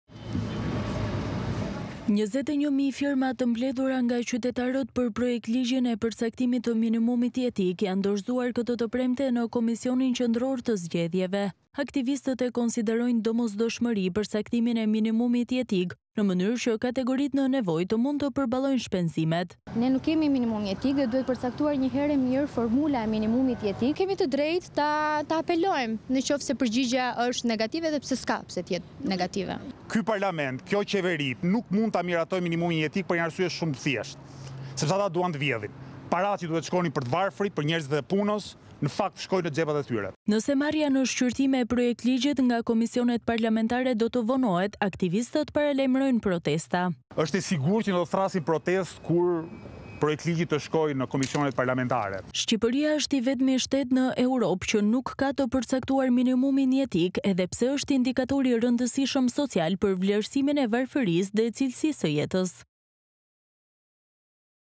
21 mijë firma për minimumin jetik dorëzohen në KQZ, aktivistët: Miratimi është domosdoshmëri
21 mijë firma për minimumin jetik dorëzohen në KQZ, aktivistët: Miratimi është domosdoshmëri